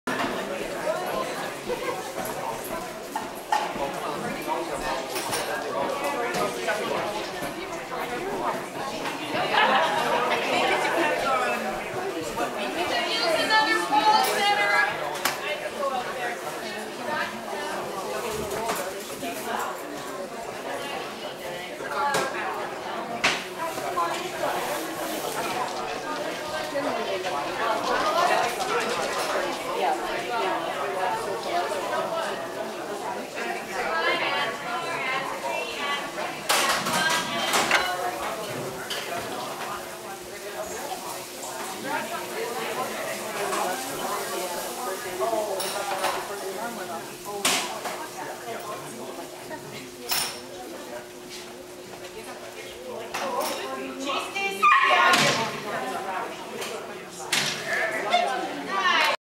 Dexter AD 1st Leg!
Little Dex finished the weekend on a good note... remembering all 12 weave poles ... in a row!